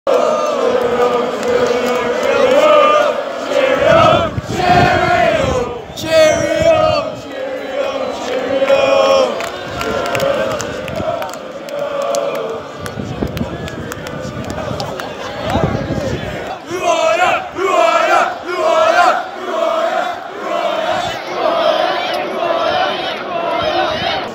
RED CARD DANIEL PHILLIPS GETS MOCKED BY IPSWICH FANS AT GILLINGHAM AWAYDAY
Ipswich fans chant and WAVE at gillingham player Daniel Phillips  after he was sent off during gillingham v lpswich match  good game but awful weather
and that awful open stand never good when it rains  IF YOU BEEN THEIR YOU KNOW WHAT I MEAN ACCRINGTON STANLEY ALSO BAD